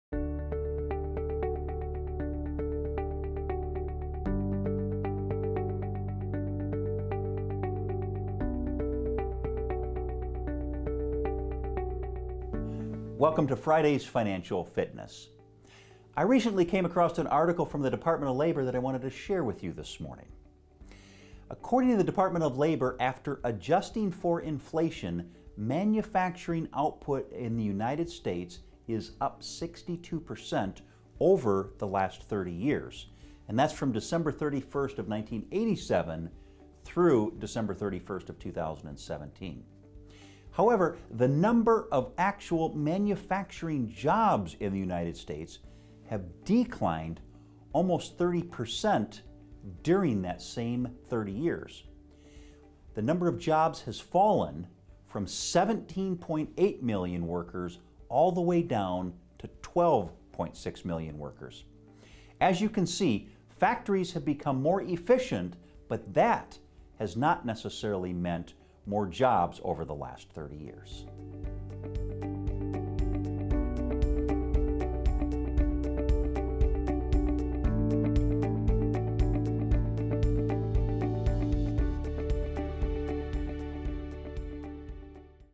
How Technology has Changed our Workforce
Dan digs into what advances in technology have meant over the last 30 years - both in terms of dramatic increases in manufacturing output, and dramatic decreases in factory workers.